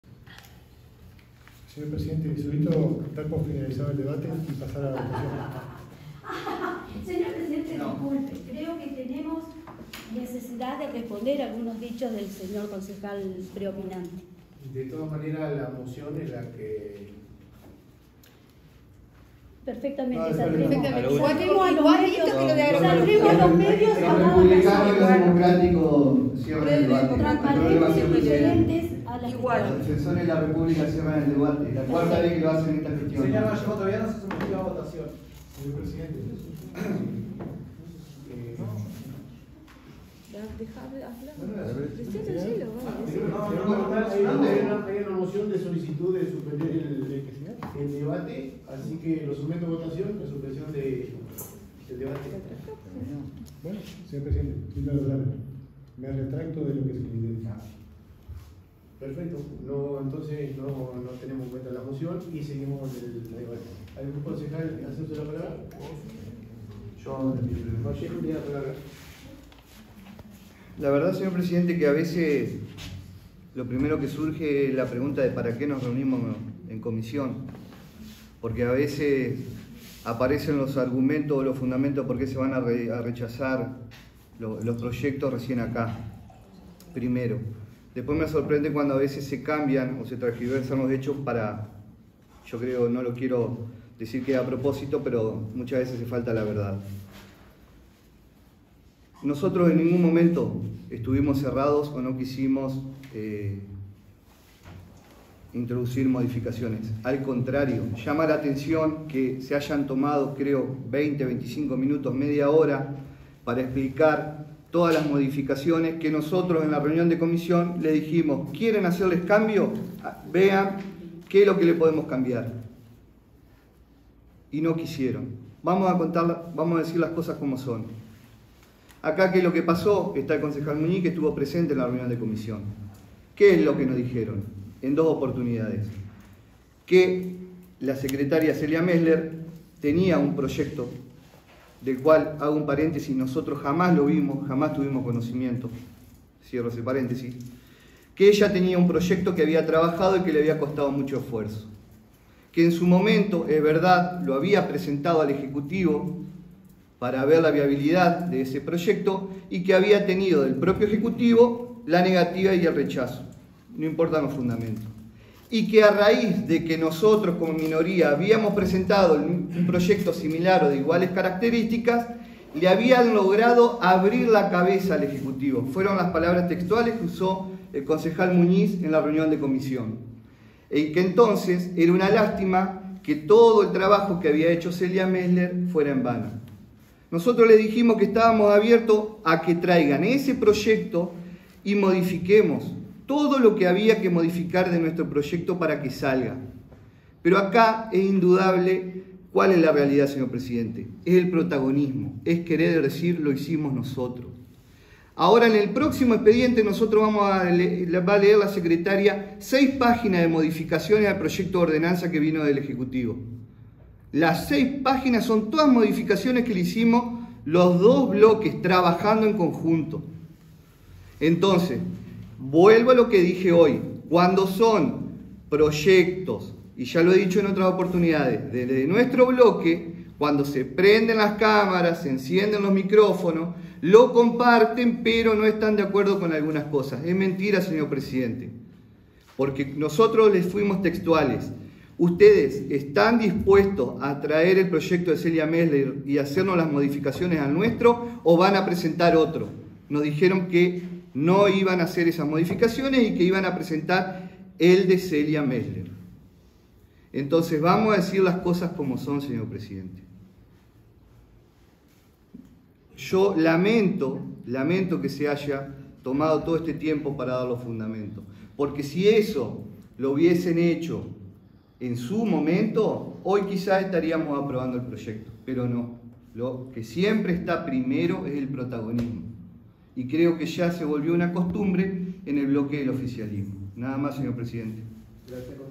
Oficialistas y opositories se cruzaron por un proyecto en apoyo a emprendedores locales
Promediando la sesión de este jueves hubo cortocircuitos entre integrantes del bloque del Frente Justicialista Creer Entre Ríos y de Juntos por el Cambio, cuando a instancias del Concejal Gabriel Frank se estuvo a punto de interrumpir un debate sobre la creación de un programa que impulse y apoye a los emprendedores locales. Frank retiró la moción de orden y la cuestión siguió siendo discutida, con un final conocido antemano, que era el rechazo al proyecto presentado por la minoría.